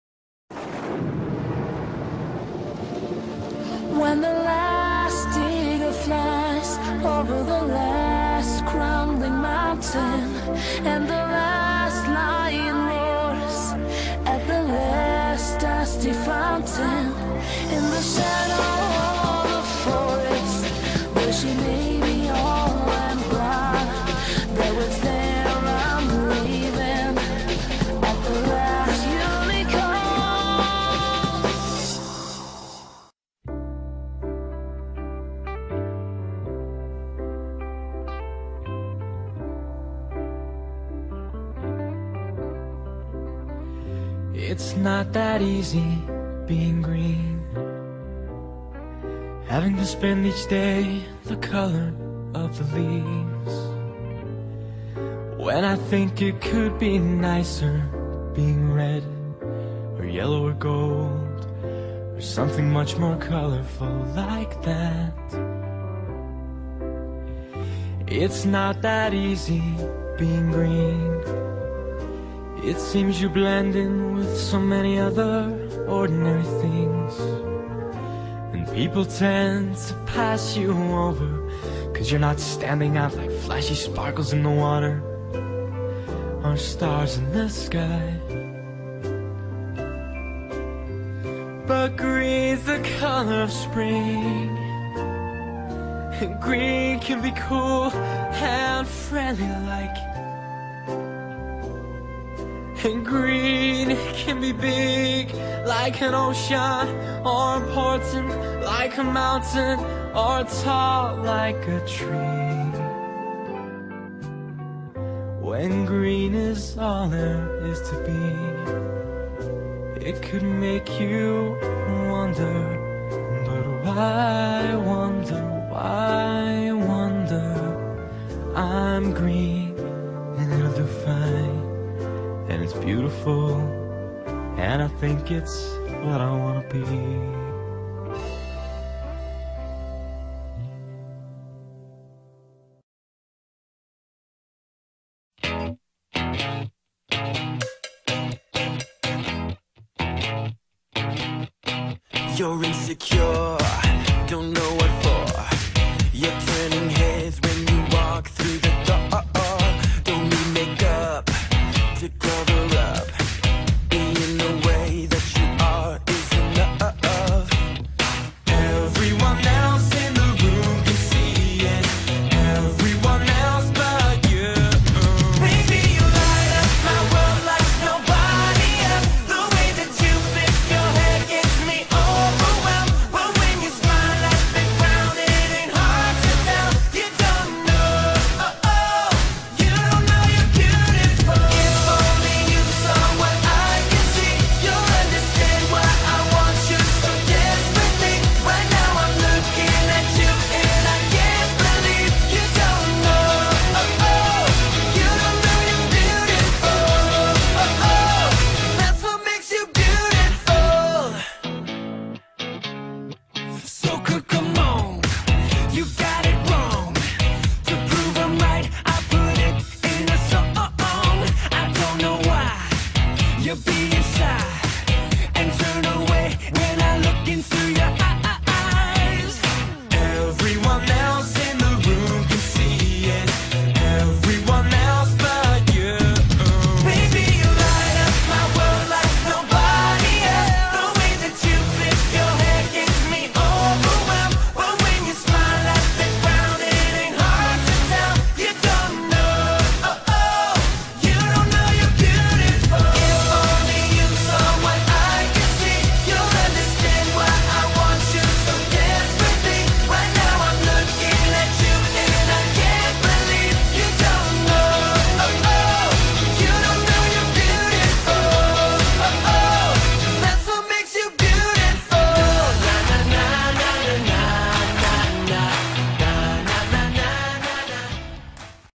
Damian McGinty Speedpaint
Damian McGinty belongs to himself.

Anyhoo, aaahhhh I hope this isn't weird! ;w; But I've been wanting to make this wonderful man some artwork for a while now. He's an amazing singer, a talented performer, and an all around good guy.

If you haven't heard his music (other than what's in the video), then go! Go my watchers go! Find it and listen~! 8D